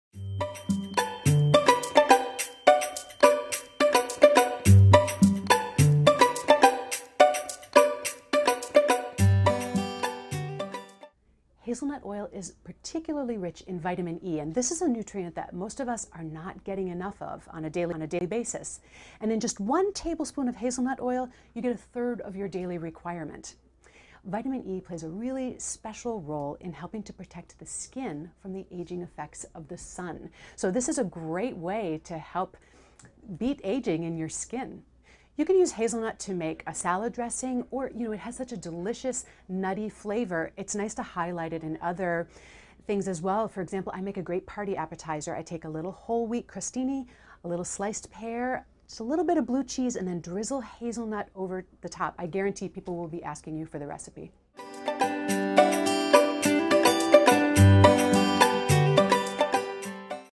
What Are the Health Benefits of Hazelnut Oil? with Nutrition Diva, Monica Reinagel
Have you ever heard of hazelnut oil? Well, it's loaded with nutritious value.  Quick and Dirty Tips presents "What Are the Health Benefits of Hazelnut Oil?" with Nutrition Diva, Monica Reinagel.  In this video, learn what hazelnut oil is good for and a few tips of how to use it to maximize its health benefits.